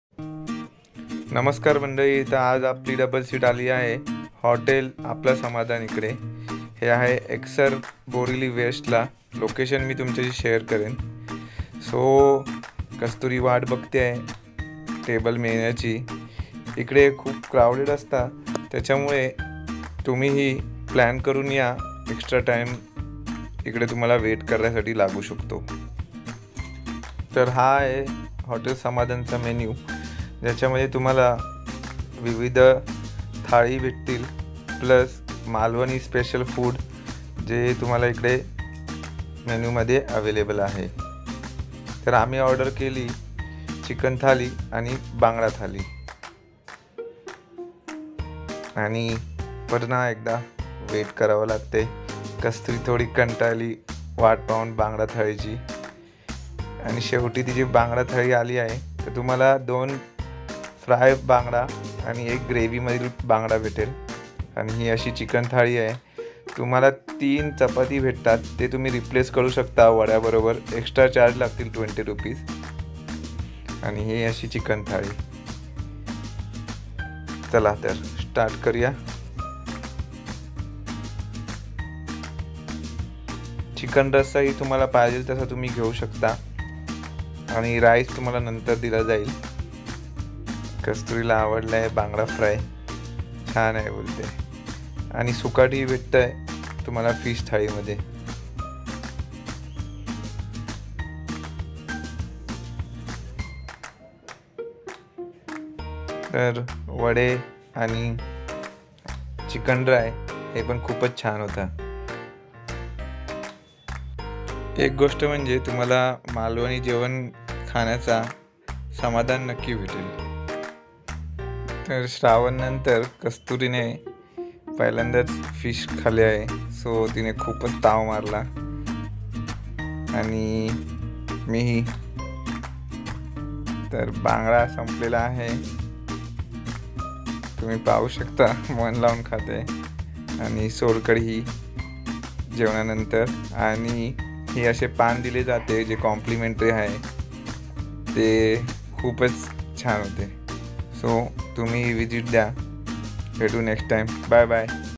014 Malvani Food at Apala Samadhan Hotel, Eksar Borivali West
one of best place for Malvani food at Eksar, Borivali West, Mumbai
We Ordered Bangada Thali and Chicken Thali